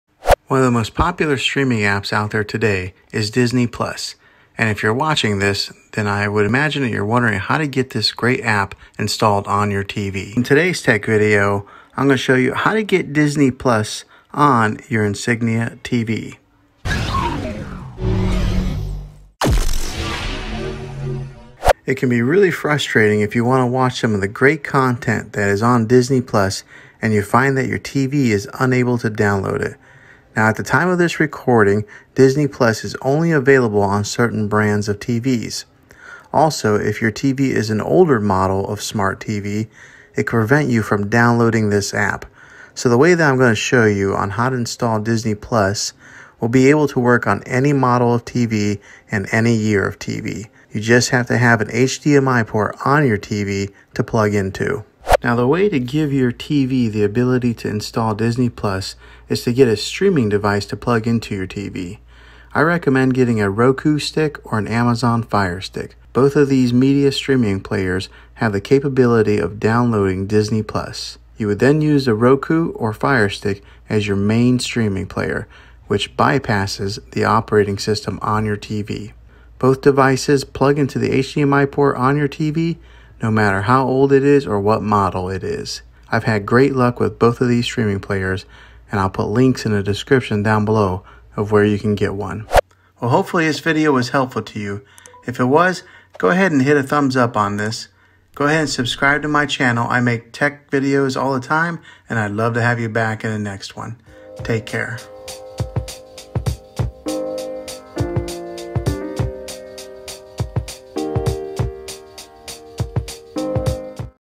How to get Disney Plus on any Insignia TV
This video will show you how to get Disney Plus on any Insignia TV.